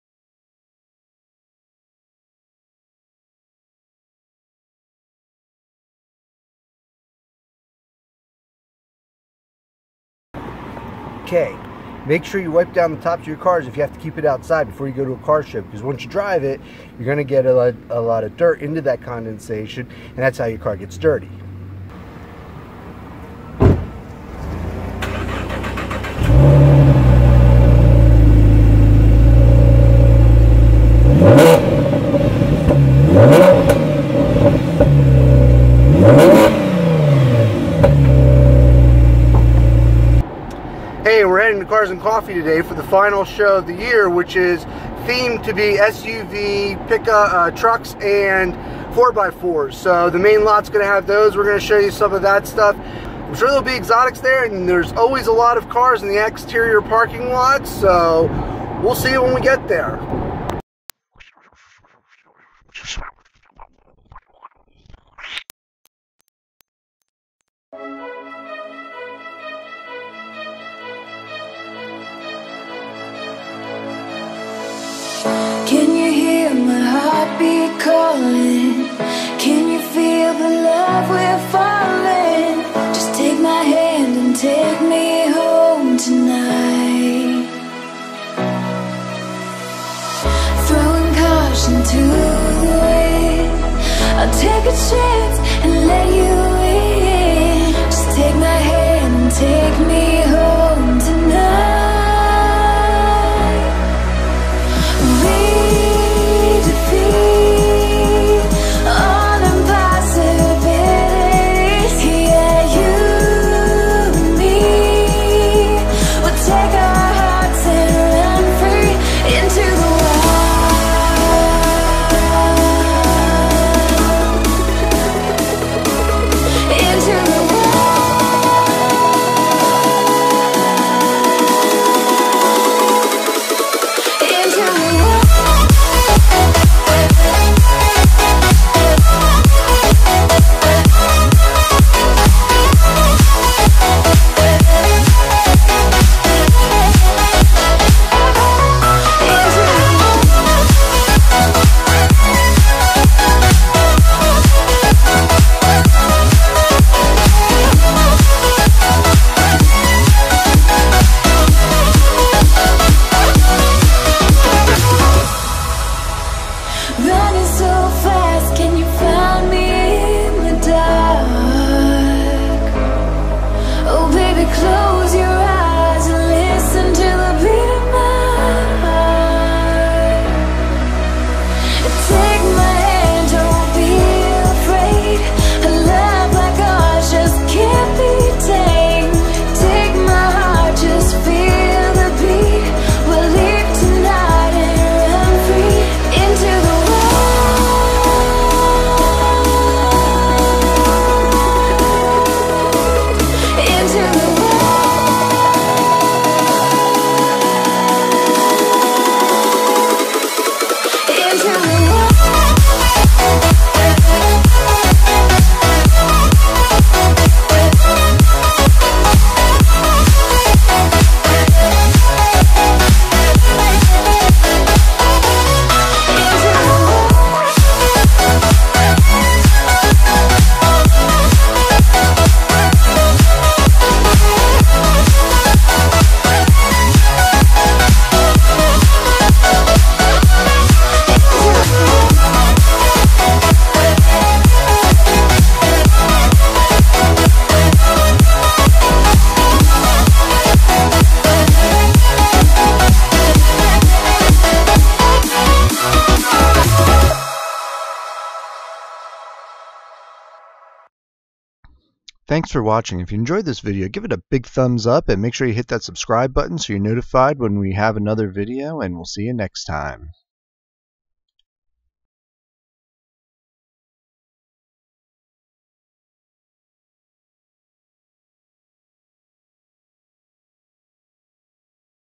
Cars and Coffee - Steel Stacks - Trucks
Take a look at the last Cars and Coffee -  Lehigh Valley  at the Bethlehem Steel Stacks for the SUV, Trucks and 4X4 themed show.

Kodak Pixpro SP360 4K
Canon 6D
Canon 7D
Canon 24-70 f2.8 L Lens
Sigma 70-200 f2.8
Adobe Creative Suite